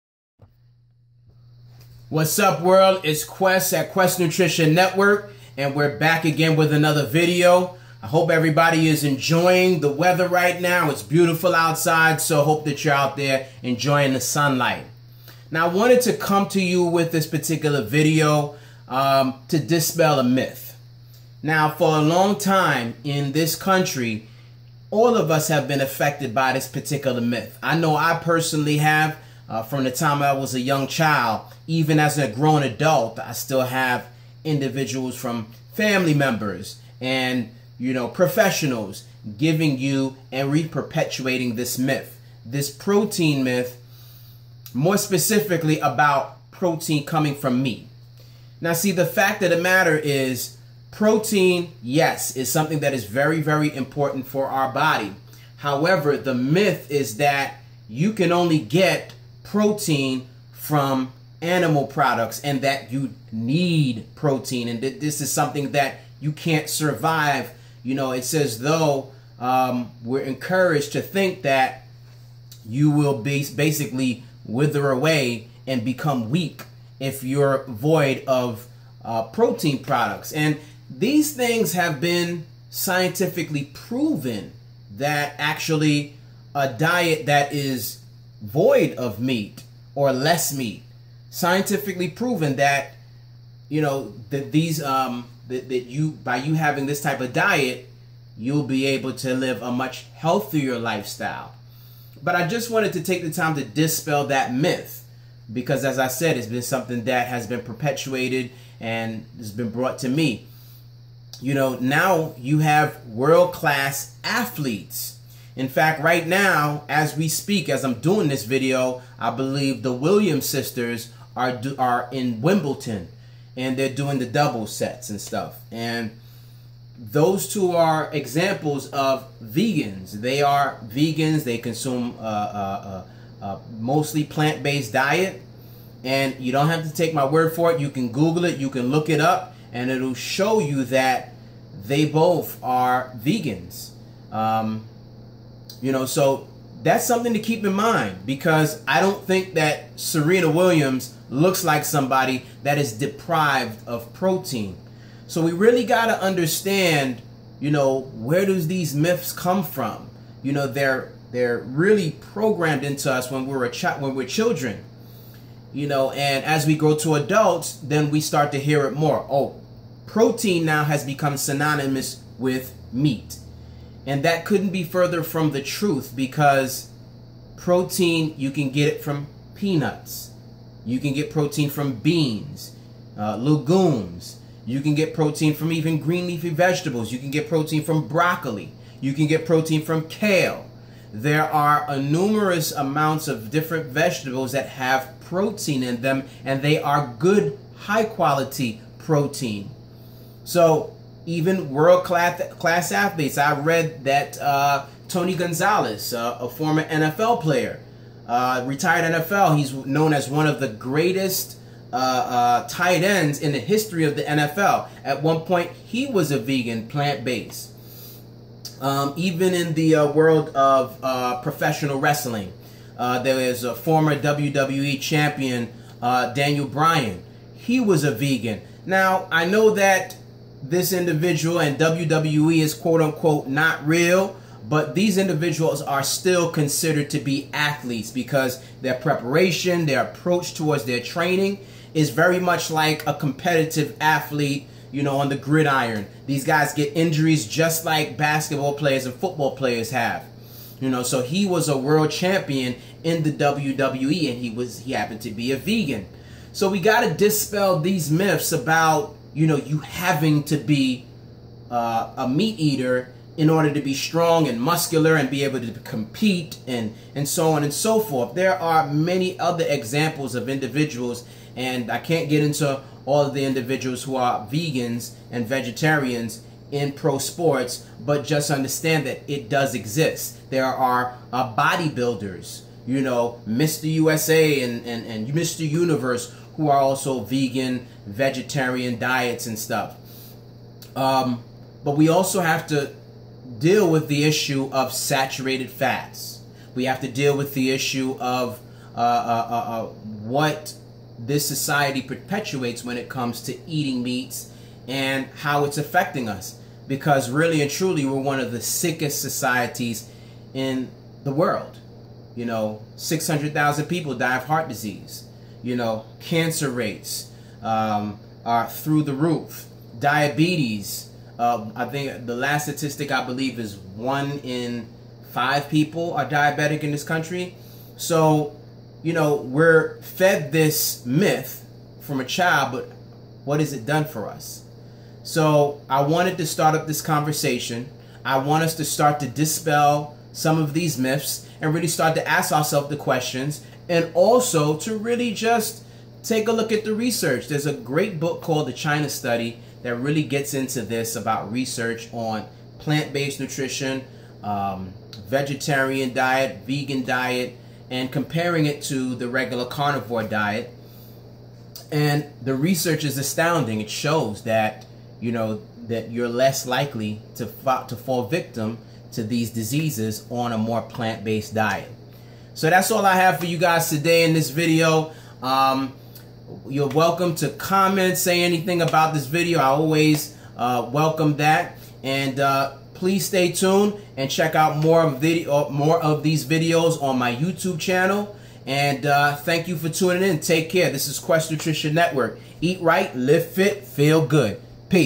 Dispelling the myth of animal protein
Protein is very important to our diets however it is important that we recognize that where we choose to get our protein has a great impact on our overall health. In this video my goal is to attempt to start the conversation about the myths of animal protein.